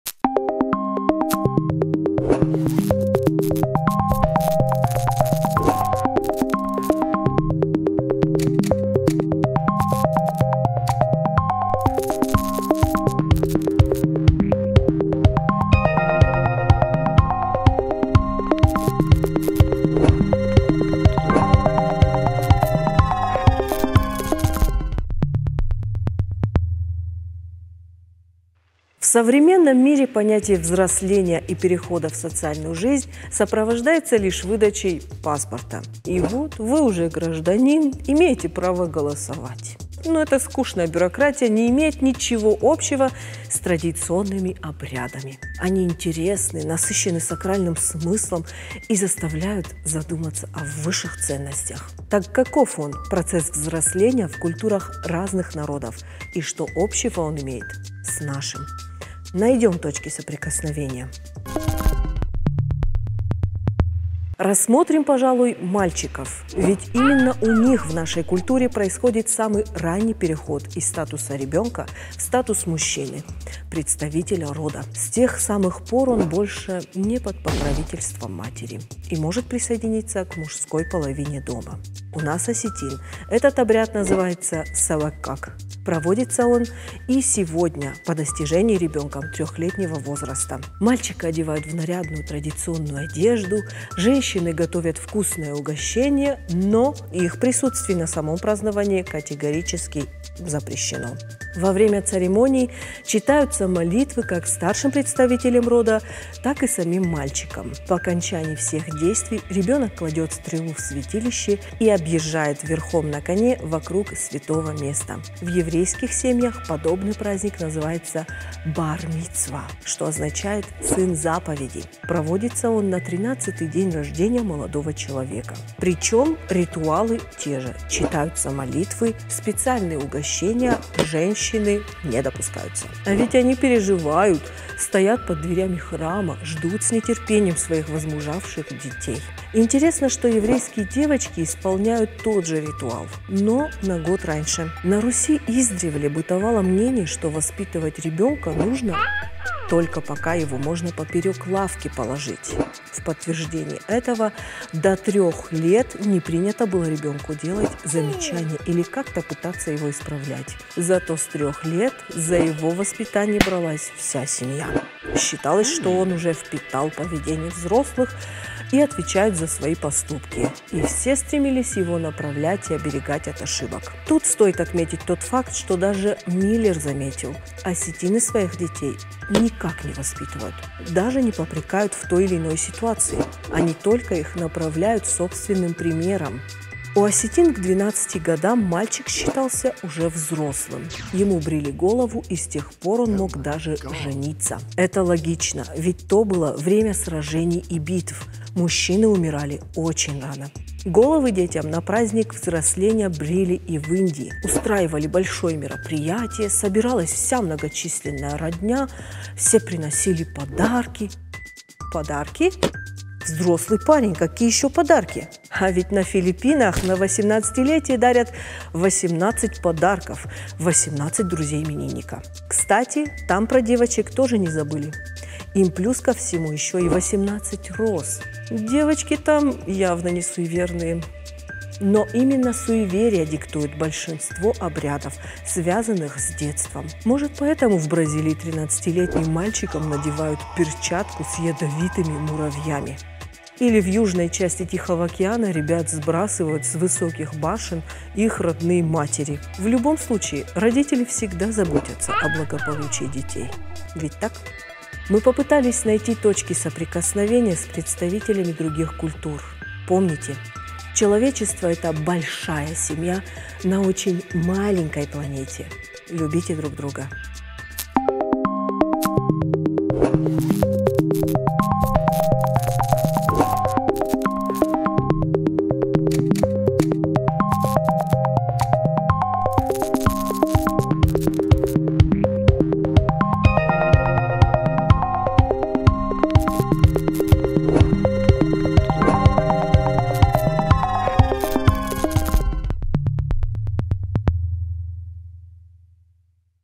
Взросление I Точки
В современном мире понятие взросления и перехода в социальную жизнь сопровождается лишь выдачей паспорта. И вот, вы уже гражданин, имеете право голосовать. Эта скучная бюрократия не имеет ничего общего с традиционными обрядами. Они интересны, насыщены сакральным смыслом и заставляют задуматься о высших ценностях. Так каков он – процесс взросления в культурах разных народов и что общего он имеет с нашим. Найдем точки соприкосновения.